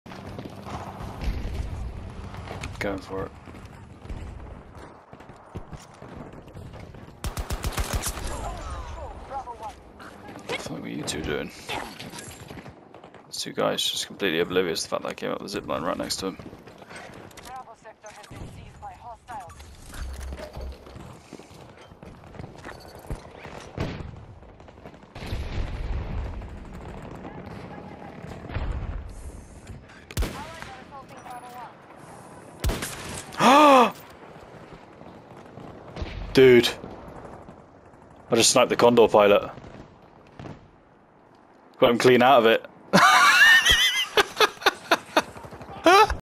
I said I was "going for it!" & "IT" WENT WELL 🤣🔥
Using this zipline was certainly risky, but paid off beautifully 💪

I genuinely surprised myself with the snipe...if you couldn't tell LOL 😂